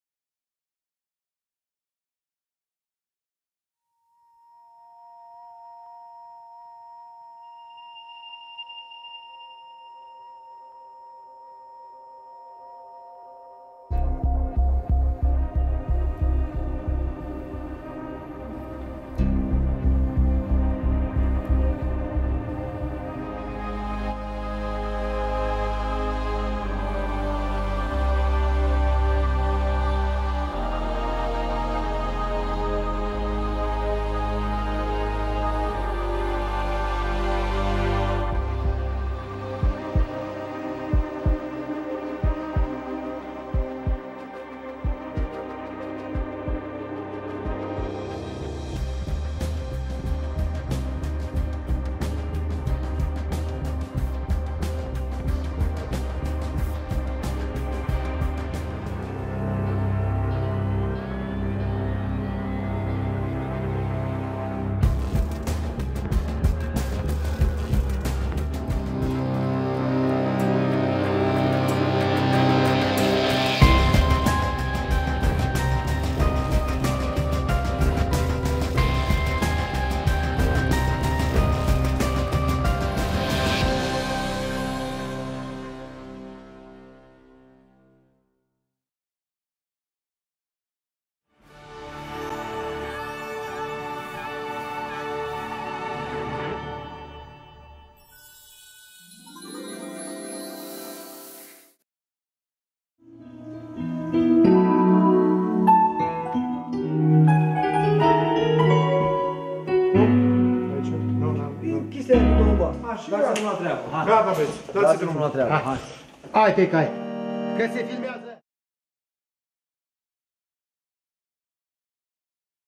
Trailer Formatia Mondial, Restaurant Gospodine Piatra Neamt
"O seară minunată e sărbătoare și răsună muzica la casa cu omenie visez la o mândră mireasă... super formație și locație. — with Uta Daniel, Florin Mihut, Vasyle Plescan, Cercul Gospodinelor Minerva, Formatia Mondial Neamt la Cercul Gospodinelor.